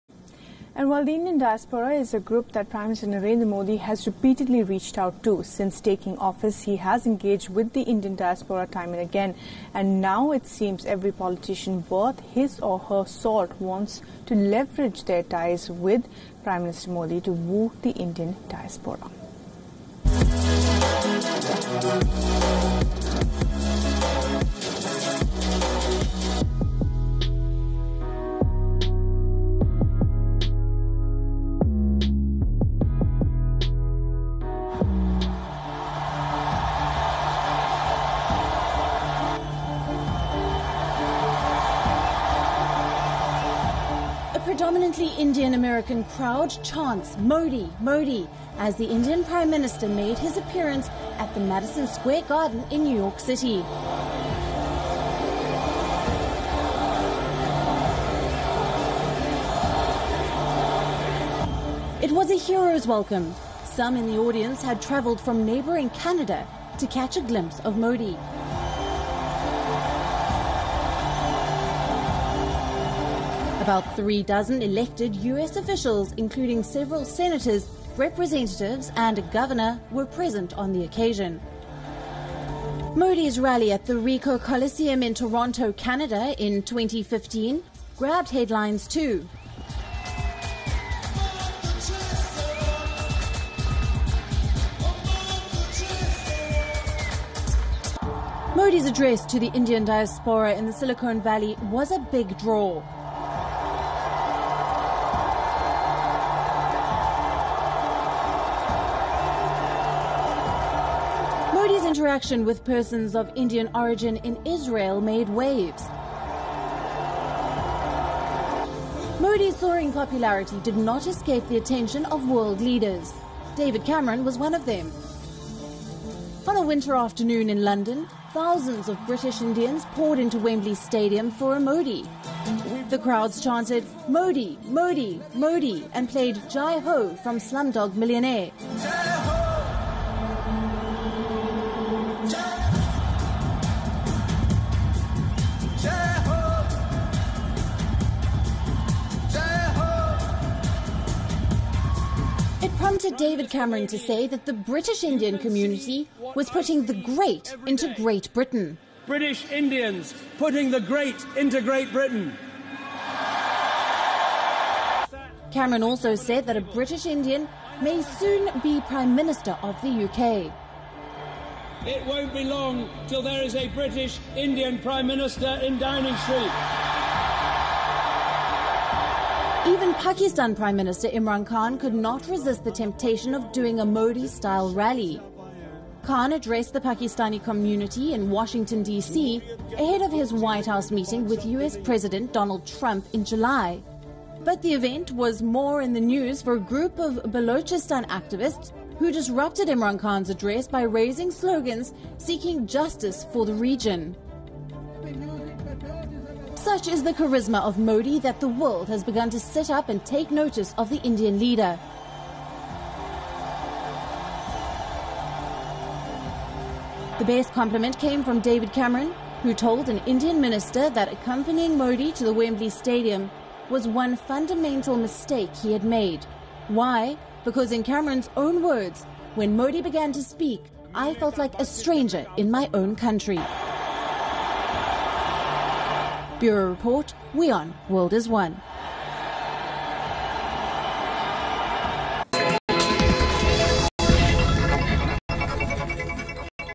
Leaders want to ride on Modi's Charisma
PM Modi takes the world by storm. Leaders want to ride on Modi's Charisma